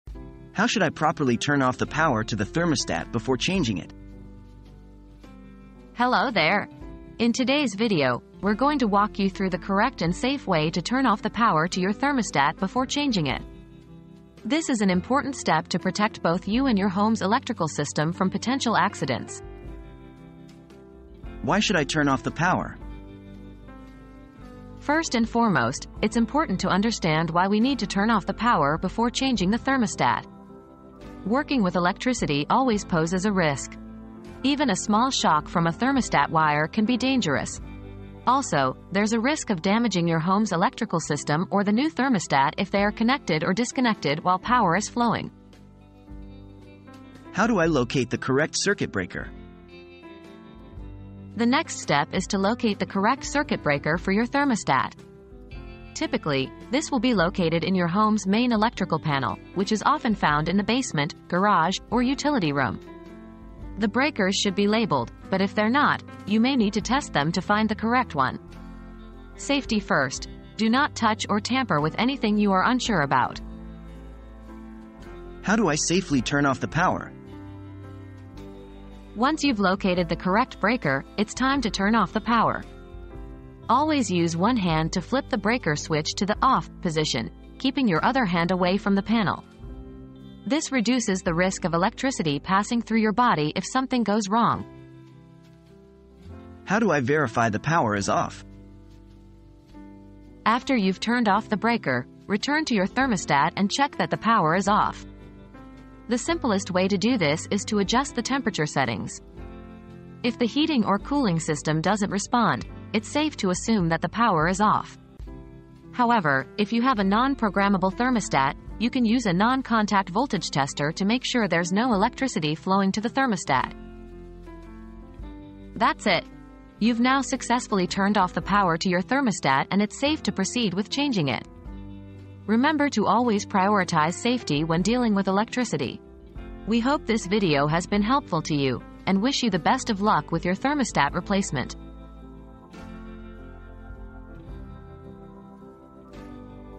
How should I properly turn off the power to the thermostat before changing it?
How to Safely Power Off Your Thermostat Before Replacement • Learn the correct and safe way to turn off the power to your thermostat before changing it in this informative video. Discover why it's important to power off, how to locate the correct circuit breaker, safely turn off the power, and verify that it's off. Protect yourself and your home's electrical system with these essential steps!

00:00 • How should I properly turn off the power to the thermostat before changing it?
00:21 • Why Should I Turn Off the Power?
00:47 • How Do I Locate the Correct Circuit Breaker?
01:15 • How Do I Safely Turn Off the Power?
01:36 • How Do I Verify the Power is Off?